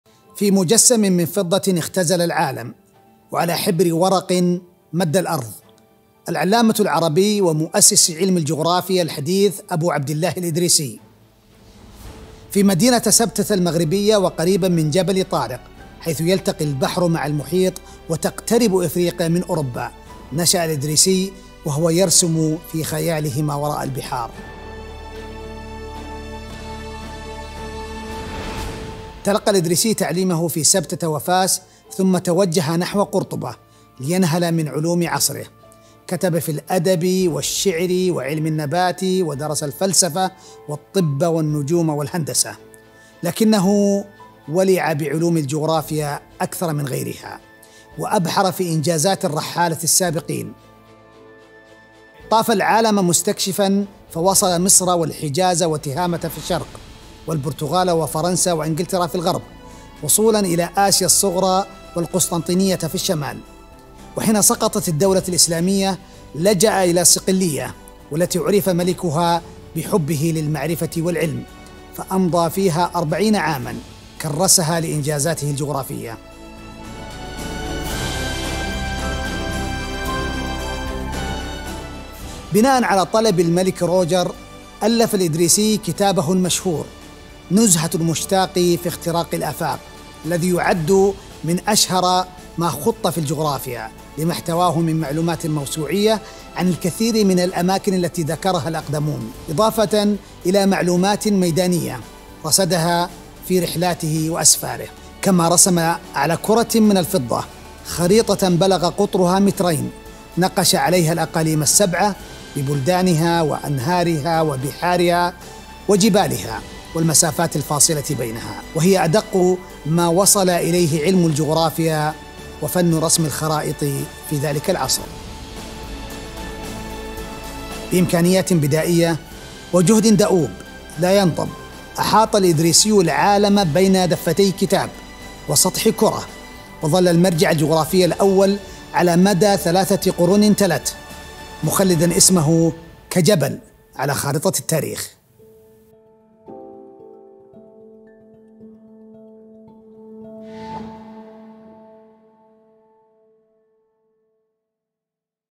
ألهم عالمك مع الإدريسي
سلسلة حلقات تتناول أهم الشخصيات التي ألهمت العالم بفنها وعلمها وإبداعها وتركت بصمة خالدة دونتها صفحات التاريخ، تتناول هذه الحلقة العلامة العربي ومؤسس علم الجغرافيا الحديث أبو عبدالله الإدريسي، وكيف استطاع أن يرسم في خياله ما وراء البحار وما هو سر ولعه بالجغرافيا.

مرحبًا بكم في قناة مركز الملك عبدالعزيز الثقافي العالمي (إثراء) على اليوتيوب.

Welcome to the King Abdulaziz Center for World Culture (Ithra) YouTube channel.